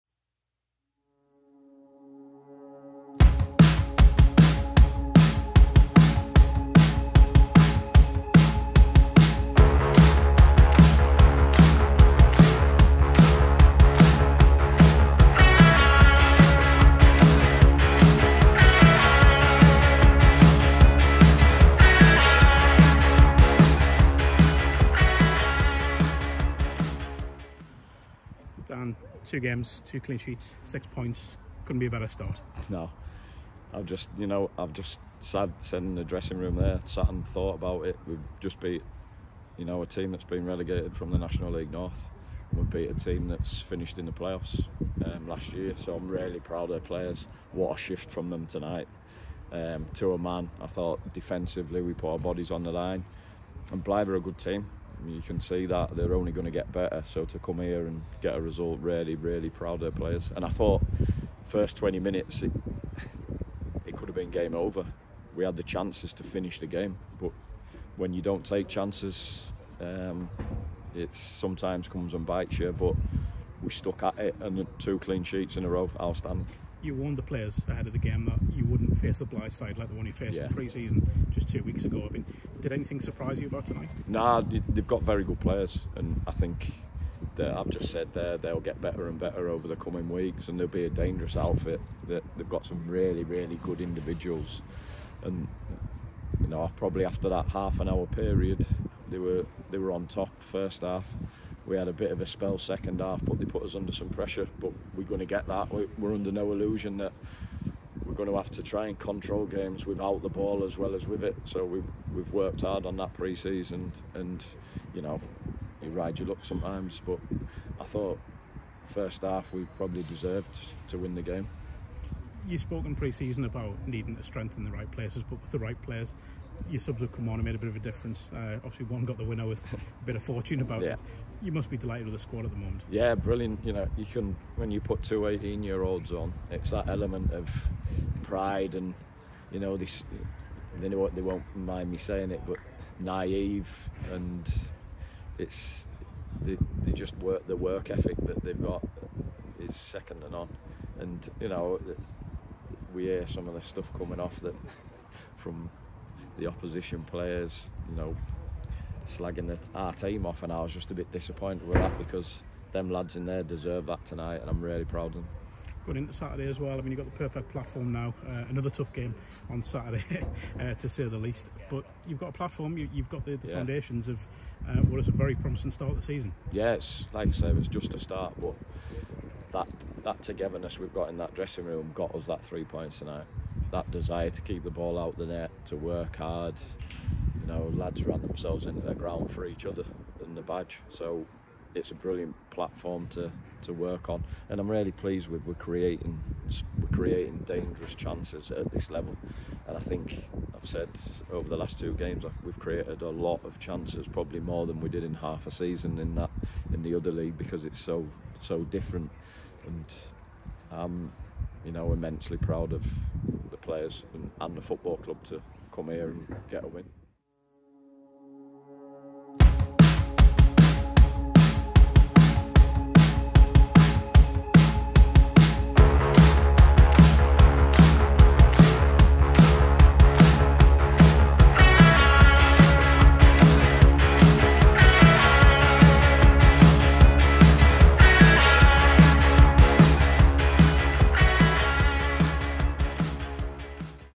Daniel Moore praises Hebburn Town after win at Blyth Spartans
Hebburn Town manager Daniel Moore gives his verdict on his side’s 1-0 win at Blyth Spartans as the Hornets made it two wins from two in the Northern Premier League Premier Division.